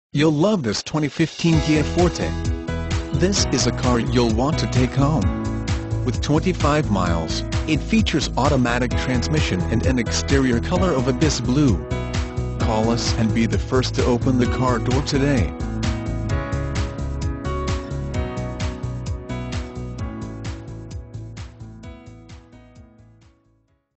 2015 KIA FORTE Austin, TX 281304
2015 KIA FORTE Austin, TX
Stock# 281304

Capitol Kia
13573 North Hwy 183

You'll love this 2015 Kia Forte. This is a car you'll want to take home. With 25 miles, it features Automatic transmission and an exterior color of Abyss Blue. Call us and be the first to open the car door today!